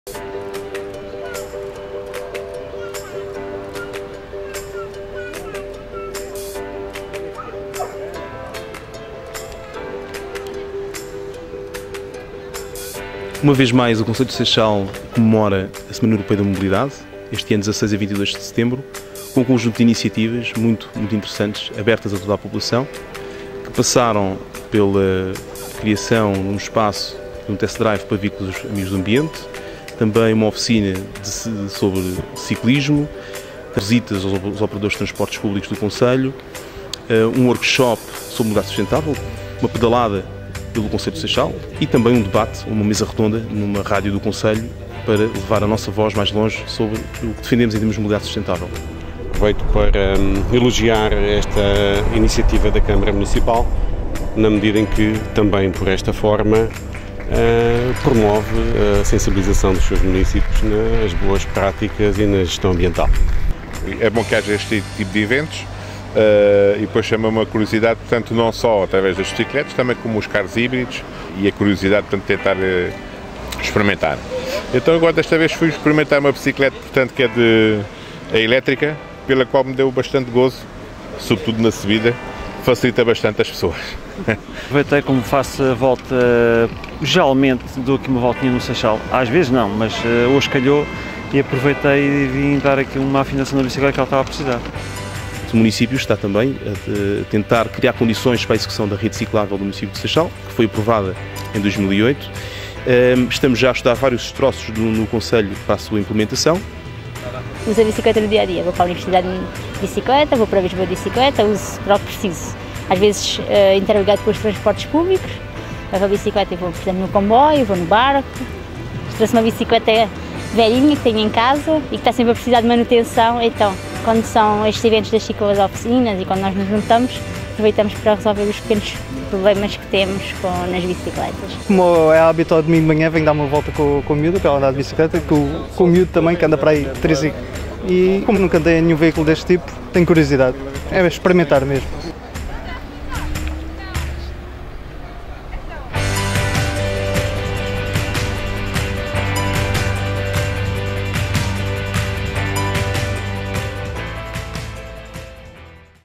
Seixal celebra Semana Europeia da Mobilidade
O Município do Seixal assinalou, de 16 a 22 de setembro, a Semana Europeia da Mobilidade, sob o lema «Avançando na direção certa». A iniciativa abordou de forma criativa a mobilidade urbana, a segurança rodoviária e os transportes alternativos, com a realização de um workshop e uma mesa redonda sobre estas temáticas, assim como test drives de veículos amigos do ambiente, uma pedalada pela Baía do Seixal e até uma Cicloficina de reparação e manutenção de bicicletas, entre outras atividades. O objetivo foi propor a munícipes e visitantes alternativas sustentáveis de mobilidade urbana.